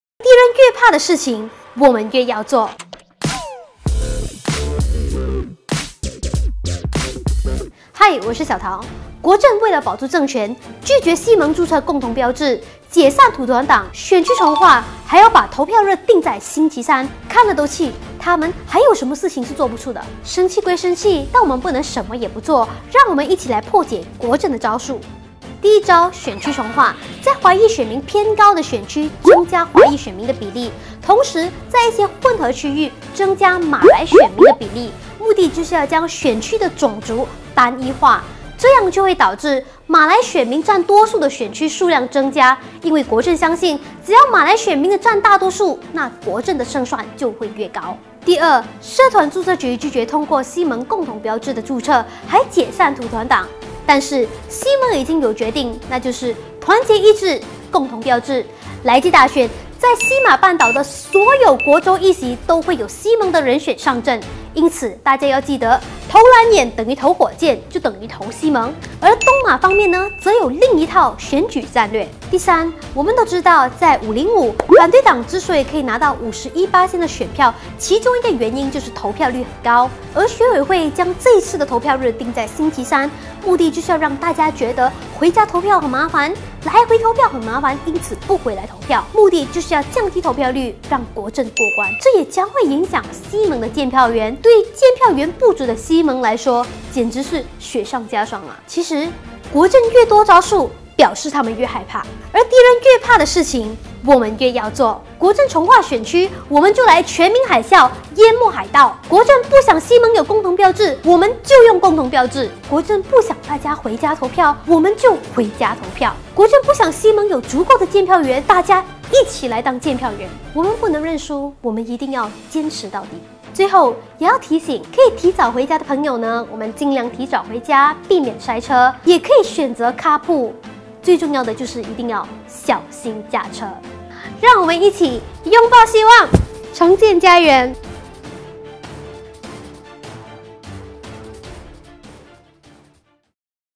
破解国阵手段的四大招数 | Ubah劲报 第14集
国阵为了保住政权，耍了不少手段。

很多网友频频问，
选区重划通过了，希盟能赢吗？
社团注册局拒绝通过希盟共同标志的注册，如何是好？
投票日定在星期三，希盟有什么对策？
我们知道大家是关心选情，也对国阵的不折手段的感到心急如焚。

看这里！让 Xiao Tao 小桃  为大家整合，告诉你破解这些手段的招数！